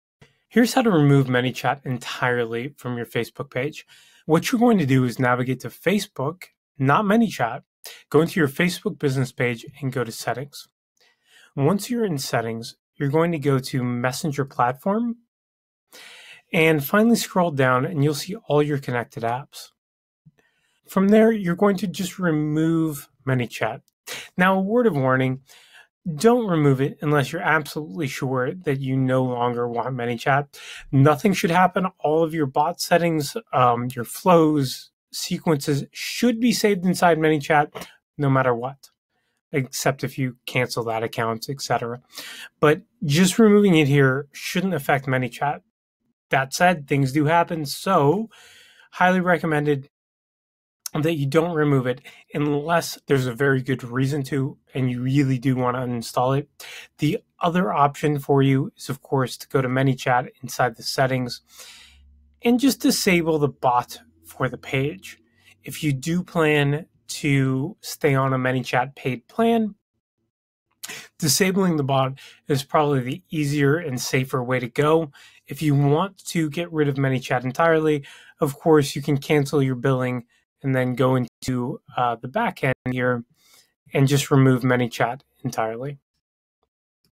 How to Remove ManyChat From Facebook
Need to remove ManyChat from your Facebook Messenger?

Here's exactly how to completely remove the ManyChat app from your Facebook business page.